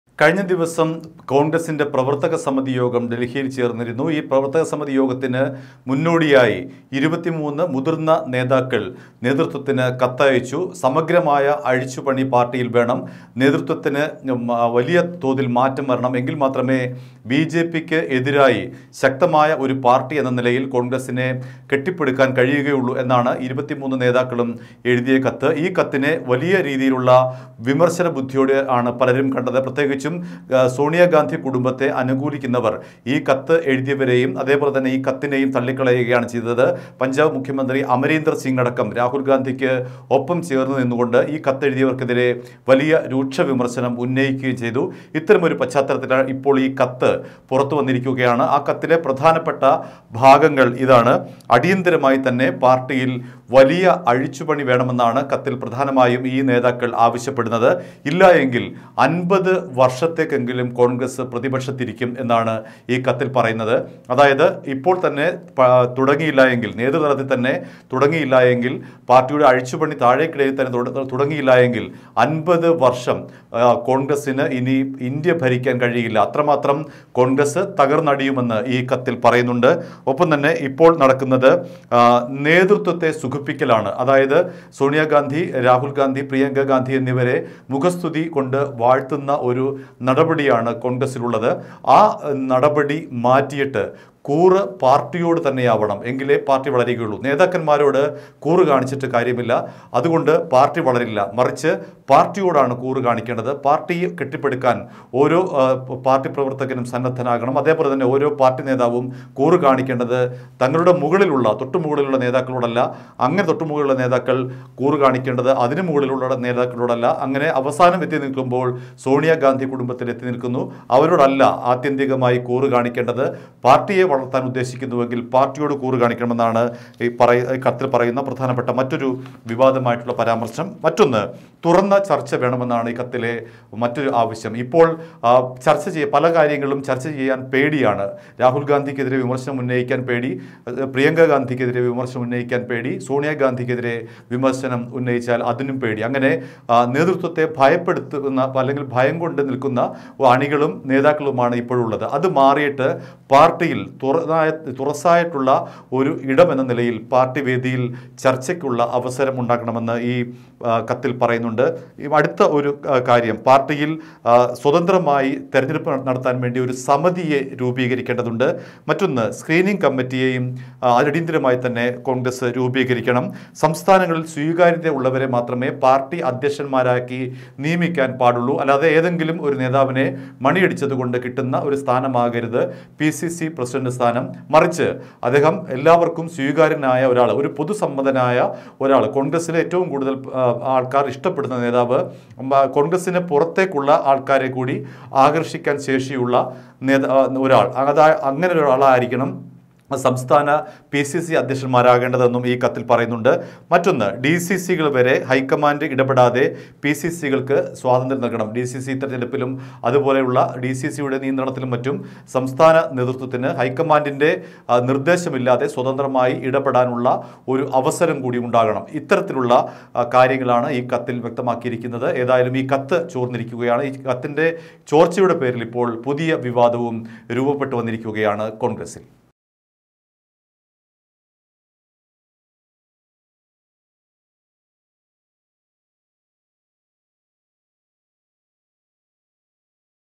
കോണ്‍ഗ്രസ് നേതൃത്വത്തിനെതിരായ കത്ത് പുറത്ത് | Complete Text Of Congress "Letter Bomb"
കോണ്‍ഗ്രസില്‍ നേതൃമാറ്റം അനിവാര്യമാണെന്ന നിലയില്‍ 23 കോണ്‍ഗ്രസ് നേതാക്കള്‍ ചേര്‍ന്ന് നേതൃത്വത്തിന് കത്ത് അയച്ചിരുന്നു. വിവാദമായ കത്ത് പുറത്തായി